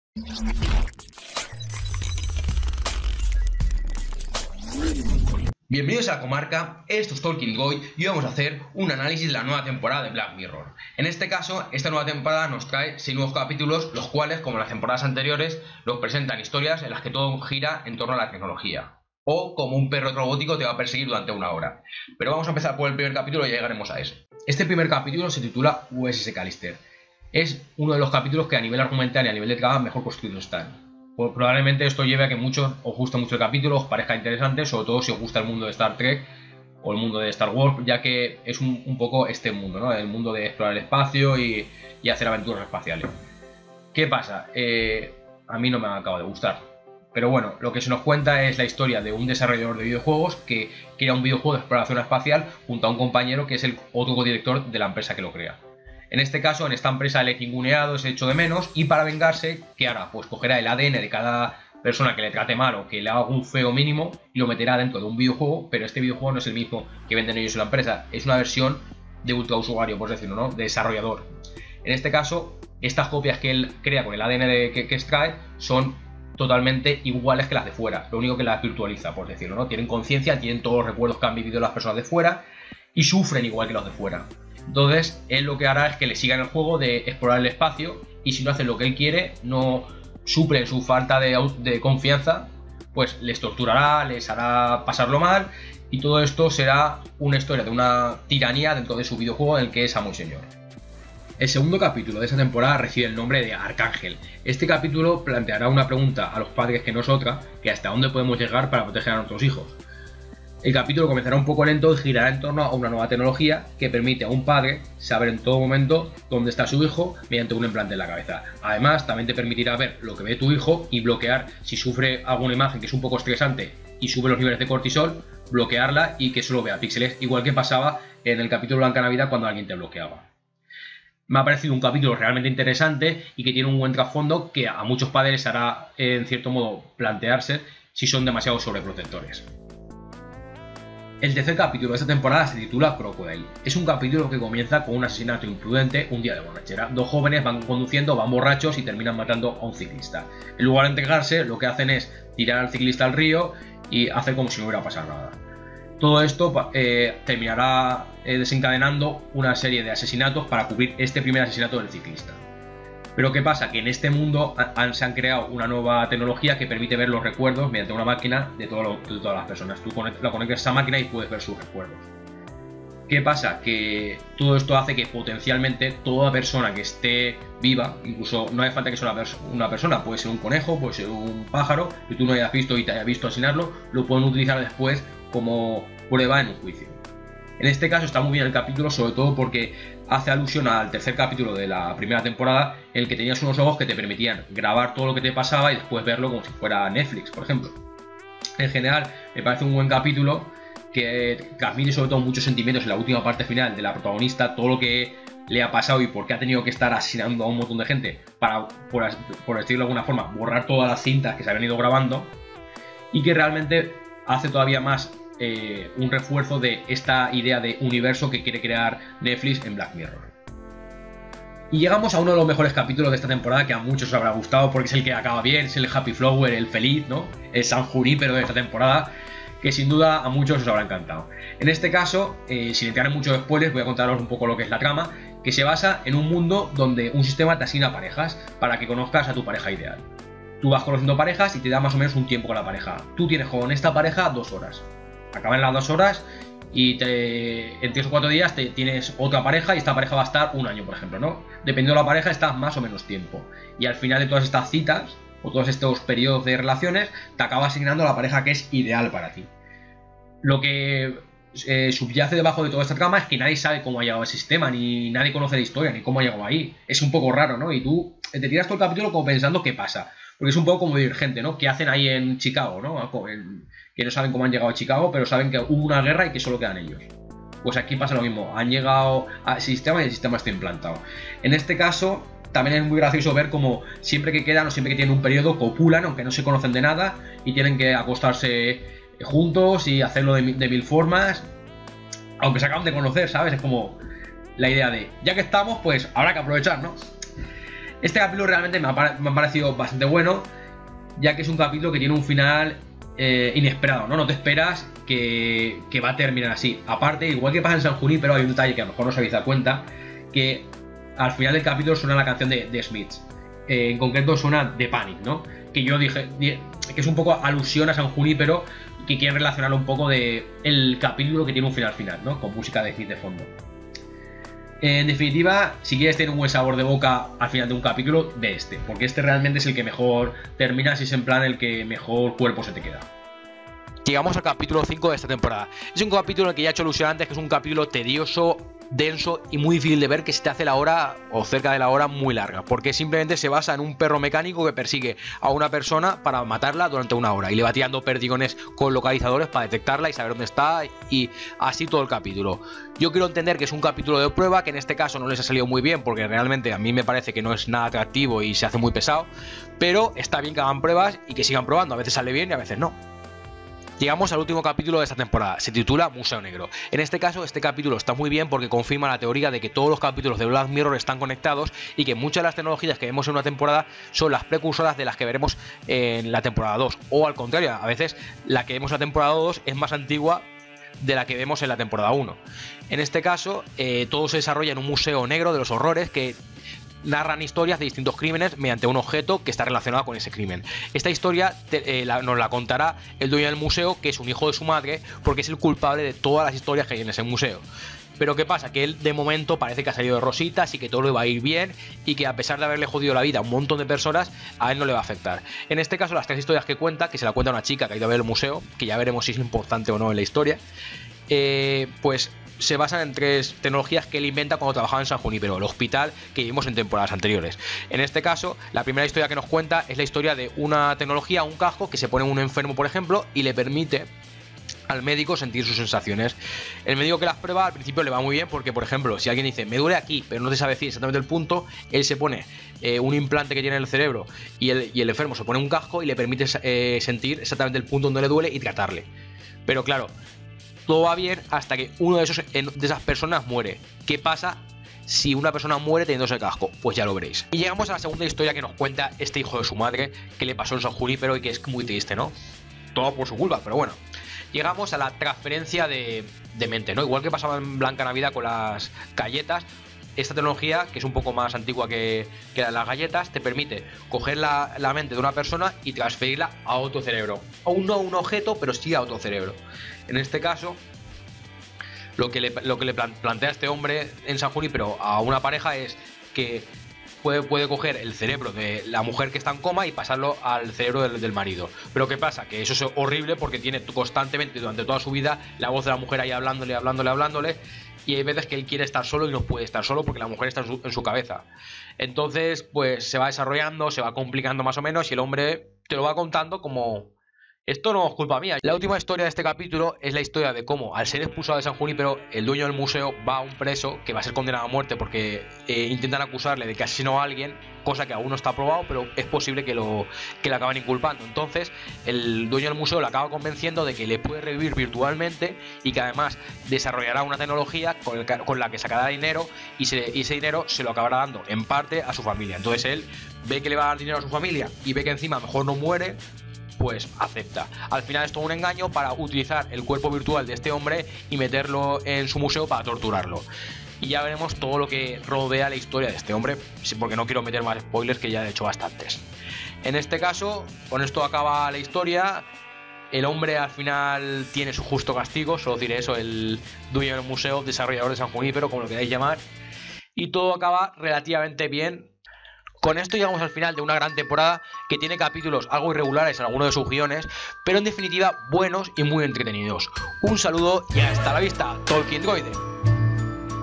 ANÁLISIS DE LA CUARTA TEMPORADA DE BLACK MIRROR (SIN SPOILERS)
Hoy os traigo este ANÁLISIS DE LA CUARTA TEMPORADA DE BLACK MIRROR (SIN SPOILERS), ante todo es mi opinión y podéis no compartirla, pero siempre con respeto, un saludo y ya tardáis en no ver la nueva temporada si no la habéis visto.